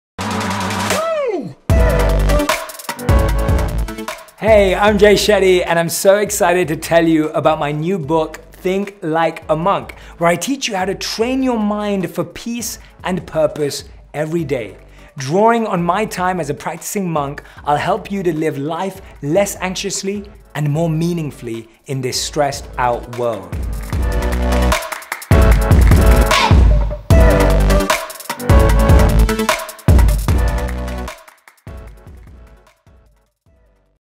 Think Like a Monk: How to Train Your Mind for Peace and Purpose Everyday
Jay Shetty, social media superstar and host of the #1 podcast ‘On Purpose', distils the timeless wisdom he learned as a practising monk into practical steps anyone can take every day to live a less anxious, more meaningful life.

The lessons monks learn are profound but often abstract. Shetty transforms them into advice and exercises we can all apply to reduce stress, improve focus, improve relationships, identify our hidden abilities, increase self-discipline and give the gifts we find in ourselves to the world. Shetty proves that everyone can – and should – think like a monk.